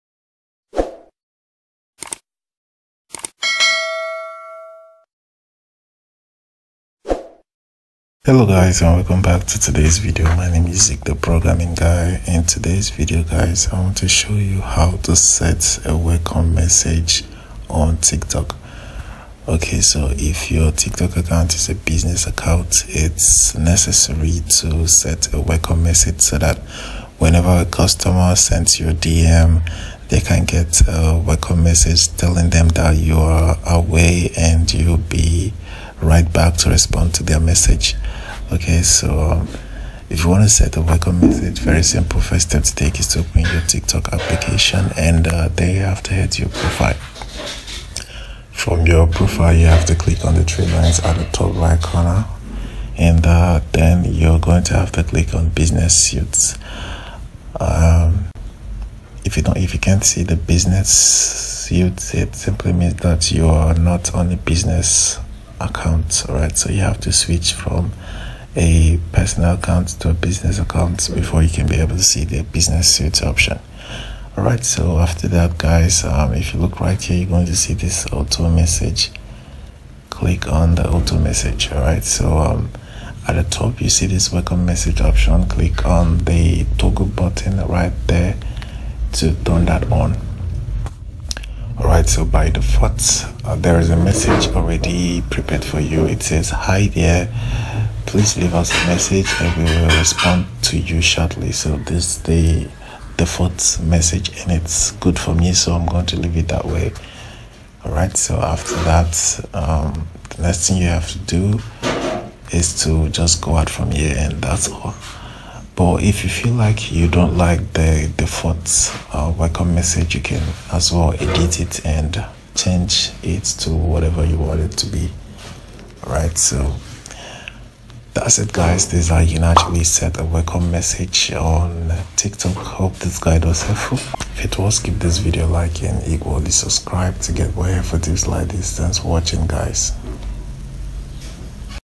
How to Set Auto Message On Tiktok
In this video. I will show you how to set welcome message on TikTok for a business account.

This video is related to :-
► how to set welcome message with mimu
► how to set welcome message in discord
► how to set welcome message with carl bot